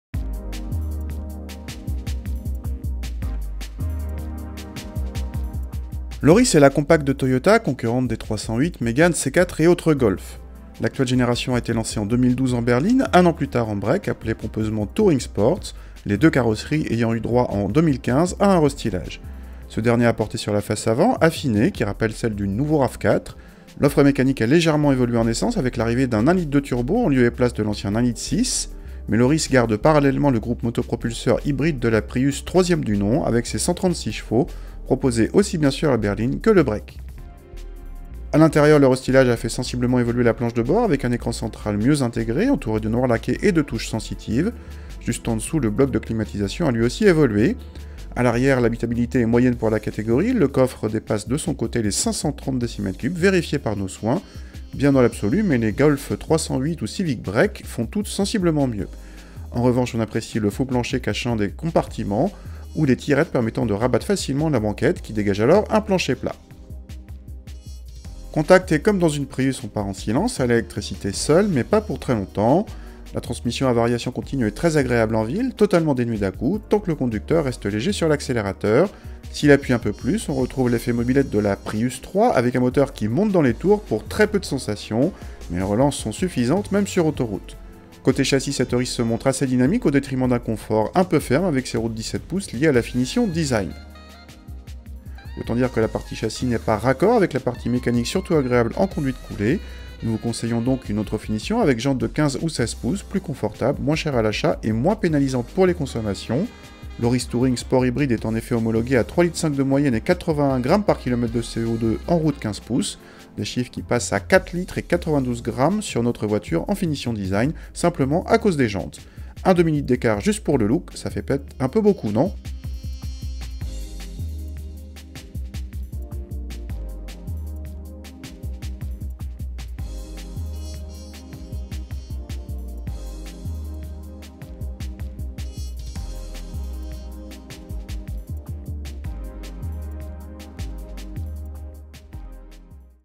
Essai Toyota Auris Touring Sports 136h Design 2016
L’Auris est la compacte de Toyota, concurrente des 308, Mégane, C4 et autres Golf. L’actuelle génération a été lancée en 2012 en berline, un an plus tard en break appelé pompeusement Touring Sports, les deux carrosseries ayant eu droit en 2015 à un restylage. Ce dernier a porté sur la face avant affinée qui rappelle celle du nouveau Rav4. L’offre mécanique a légèrement évolué en essence avec l’arrivée d’un 1,2 turbo en lieu et place de l’ancien 1,6 l, mais l’Auris garde parallèlement le groupe motopropulseur hybride de la Prius, 3e du nom, avec ses 136 ch, proposé aussi bien sur la berline que sur le break.

A l’intérieur, le restylage a fait sensiblement évoluer la planche de bord avec un écran central mieux intégré, entouré de noir laqué et de touches sensitives. Juste en-dessous, le bloc de climatisation a lui aussi évolué,. A l’arrière, l’habitabilité est moyenne pour la catégorie. Le coffre dépasse de son côté les 530 dm3, vérifiés par nos soins : bien dans l’absolu mais les Golf, 308, ou Civic break font toutes sensiblement mieux. En revanche, on apprécie le faux plancher cachant des compartiments, ou les tirettes permettant de rabattre facilement la banquette qui dégage alors un plancher plat.

Contact et, comme dans une Prius, on part en silence, à l’électricité seule, mais pas pour très longtemps. La transmission à variation continue est très agréable en ville, totalement dénuée d’à-coups, tant que le conducteur reste léger sur l’accélérateur. S’il appuie un peu plus, on retrouve l’effet mobylette de la Prius 3 avec un moteur qui monte dans les tours pour très peu de sensations. Mais les relances sont suffisantes, même sur autoroute. Côté châssis, cette Auris se montre assez dynamique au détriment d’un confort un peu ferme avec ses roues de 17 pouces liées à la finition Design.

Autant dire que la partie châssis n’est pas raccord avec la partie mécanique surtout agréable en conduite coulée. Nous vous conseillons donc une autre finition avec jantes de 15 ou 16 pouces, plus confortable, moins chère à l’achat et moins pénalisante pour les consommations : l’Auris Touring Sports hybride est en effet homologuée à 3,5 l de moyenne et 81 g/km de CO2 en roues de 15 pouces. Des chiffres qui passent à 4 litres et 92 grammes sur notre voiture en finition Design, simplement à cause des jantes. Un demi-litre d’écart, juste pour le look, ça fait peut-être un peu beaucoup, non ?